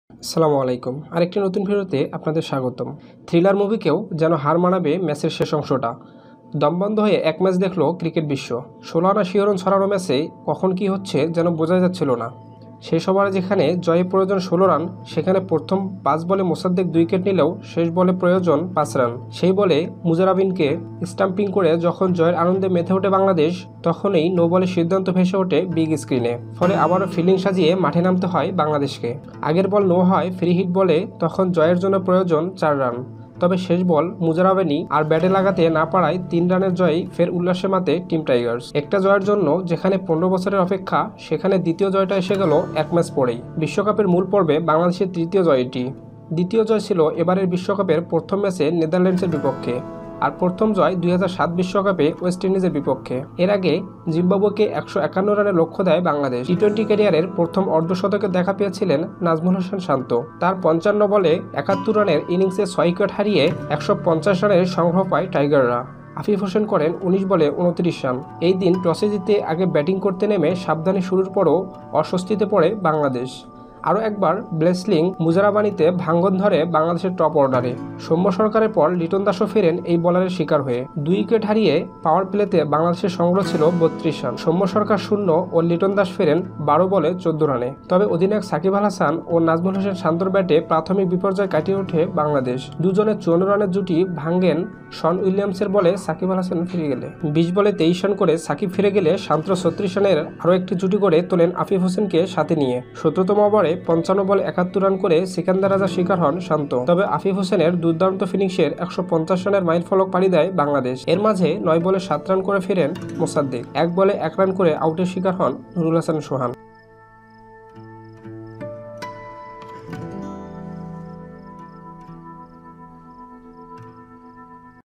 bangladeshvszimbabwe নাটকীয় ম্যাচে শ্বাসরুদ্ধ জয় পেলো বাংলাদেশ??Great Short Story??
থ্রিলার মুভিকেও যেন হার মানাবে ম্যাচের শেষ অংশটা। দমবন্ধ এক ম্যাচ দেখলো ক্রিকেট বিশ্ব। ষোলআনা শিহরণ ছড়ানো ম্যাচে কখন কী হচ্ছে যেন বুঝাই যাচ্ছিলো না।

শেষ ওভারে যেখানে জয়ে জন্য প্রয়োজন ১৬ রান, সেখানে প্রথম ৫ বলে মোসাদ্দেক ২ উইকেট নিলেও শেষ বলে প্রয়োজন হয় ৫ রান। সেই বলে মুজারাবানিকে স্ট্যাম্পিং করে যখন জয়ের আনন্দে মেতে উঠে বাংলাদেশ, তখনই নো বলের সিদ্ধান্ত ভেসে উঠে বিগ স্ক্রিনে।

ফলে আবারো ফিল্ডিং সাজিয়ে মাঠে নামতে হয় বাংলাদেশকে। আগের বল নো হওয়ায় ফ্রি হিট বলে তখন জয়ের জন্য প্রয়োজন হয় ৪ রান। তবে শেষ বল মুজারাবানি আর ব্যাটে লাগাতে না পারায় ৩ রানের জয় নিয়েই ফের উল্লাসে মাতে টিম টাইগার্স।